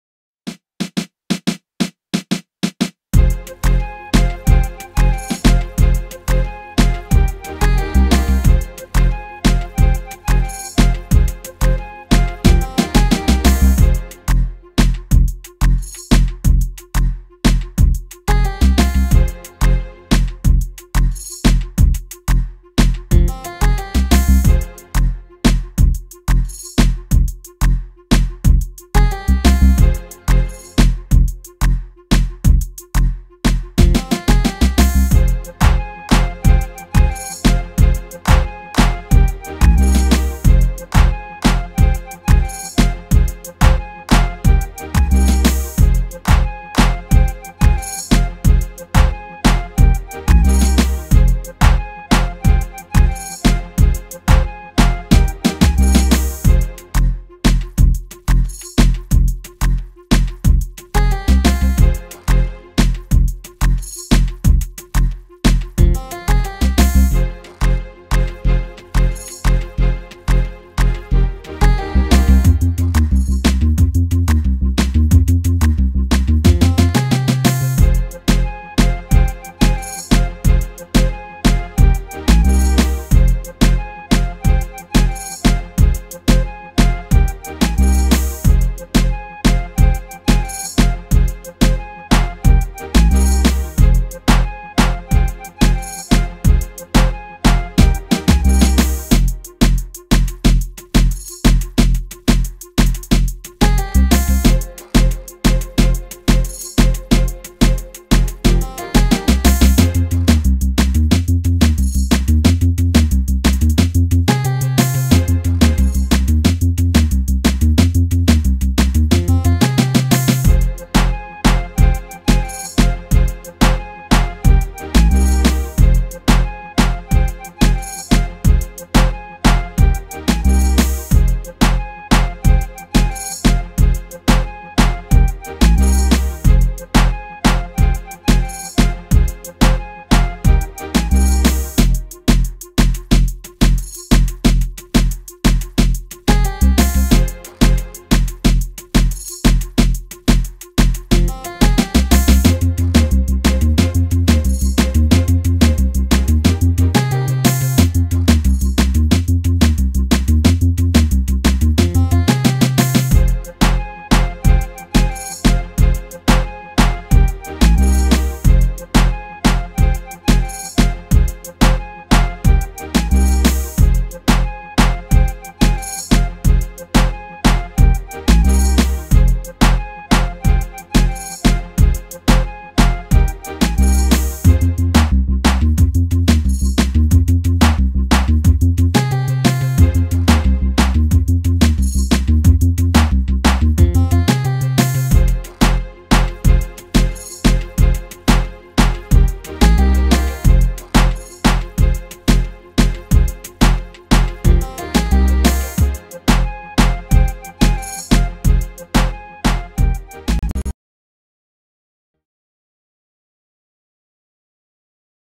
Sample Tank 3
Trying out Sample Tank 3 Making A Dance Hall Beat.